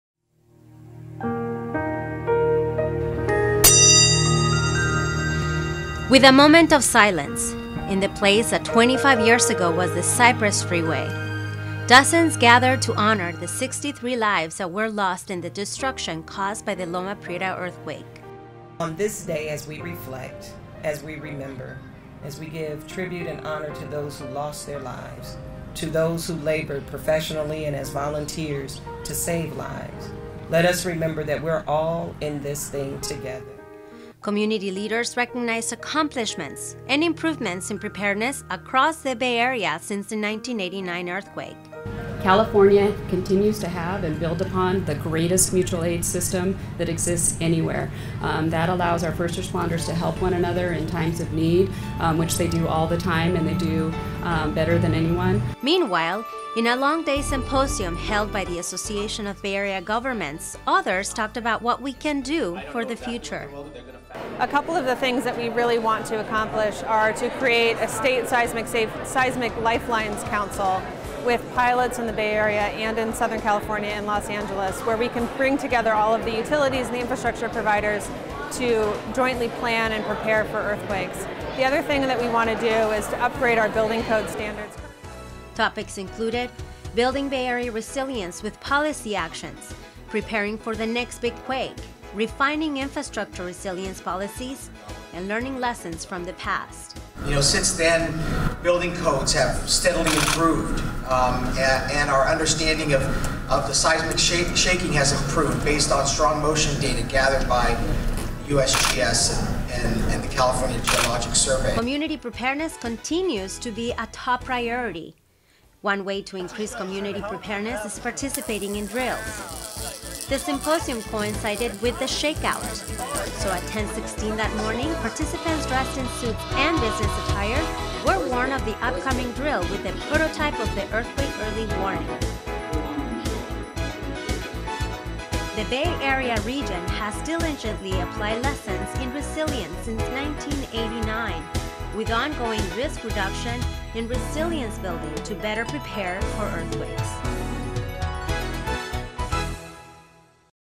Loma Prieta 25th Anniversary - Time to Reflect, Honor & Prepare
Several events were held around the Bay Area to commemorate the 25th anniversary of the Loma Prieta earthquake. The 6.9 quake hit during the World Series between the Giants and A's at 5:04 p.m. on October 17, 1989. One section of the Bay Bridge buckled, the Cypress freeway was flattened, and there was a large fire in the Marina District. Sixty-three people died and thousands were left homeless.

Cal OES participated in a few of these events to recognize the accomplishments and improvements in preparedness since the 89 quake.